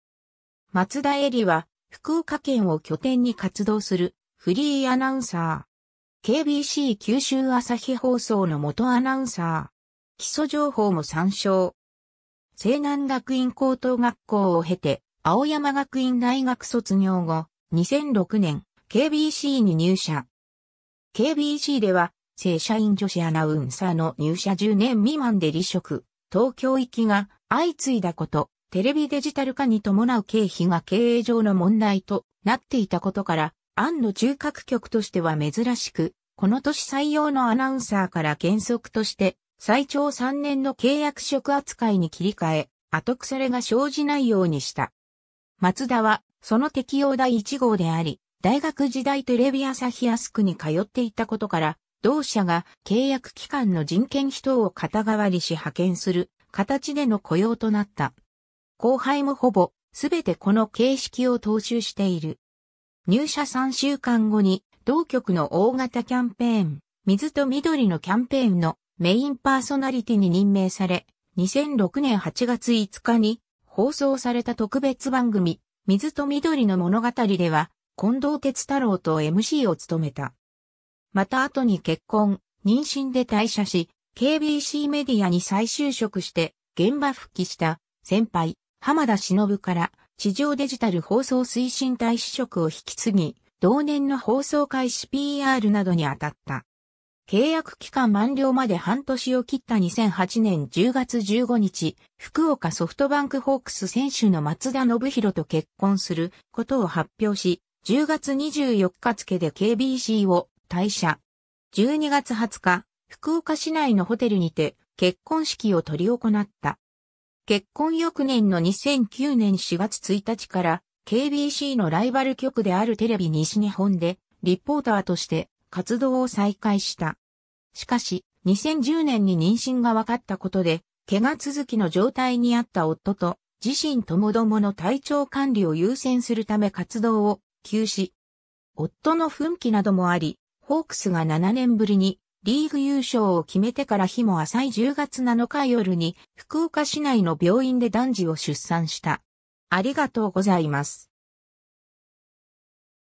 柴田恵理
松田 恵理（まつだ えり、旧姓：柴田 - しばた、 1983年8月31日 - ）は、福岡県を拠点に活動するフリーアナウンサー。
KBC九州朝日放送の元アナウンサー。
 基礎情報も参照。
西南学院高等学校を経て青山学院大学卒業後、2006年（平成18年）KBCに入社。
KBCでは正社員女子アナウンサーの入社10年未満で離職→東京行きが相次いだこと、テレビデジタル化に伴う経費が経営上の問題となっていたことから、ANNの中核局としては珍しく、この年採用のアナウンサーから原則として最長3年の契約職扱いに切り替え、後腐れが生じないようにした。
松田はその適用第1号であり、大学時代テレビ朝日アスクに通っていたことから、同社が契約期間の人件費等を肩代わりし派遣する形での雇用となった。
後輩もほぼ全てこの形式を踏襲している。
 入社3週間後に同局の大型キャンペーン「水と緑のキャンペーン」のメインパーソナリティに任命され、2006年（平成18年）8月5日に放送された特別番組「水と緑の物語」では近藤鉄太郎とMCを務めた。
また後に結婚・妊娠で退社し、KBCメディアに再就職して現場復帰した先輩・浜田しのぶから地上デジタル放送推進大使職を引き継ぎ、同年の放送開始PRなどにあたった。
 契約期間満了まで半年を切った2008年（平成20年）10月15日、福岡ソフトバンクホークス選手の松田宣浩と結婚することを発表し、10月24日付でKBCを退社。
12月20日、福岡市内のホテルにて結婚式を執り行った。
 結婚翌年の2009年（平成21年）4月1日から、KBCのライバル局であるテレビ西日本で、リポーターとして活動を再開した。
しかし、2010年（平成22年）に妊娠が分かったことで、怪我続きの状態にあった夫と自身ともどもの体調管理を優先するため活動を休止。
夫の奮起などもありホークスが7年ぶりにリーグ優勝を決めてから日も浅い10月7日夜に福岡市内の病院で男児を出産した。